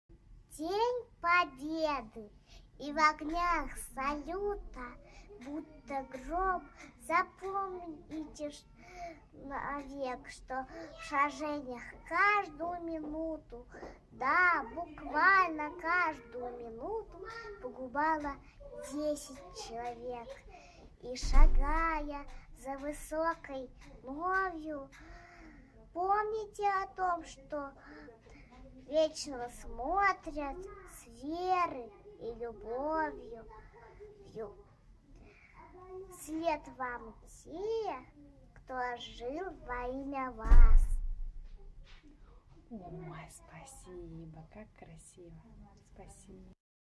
Эдуард Асадов «День Победы»
Амелия прониклась выбранным стихотворением и рассказала нам его в непринужденной обстановке.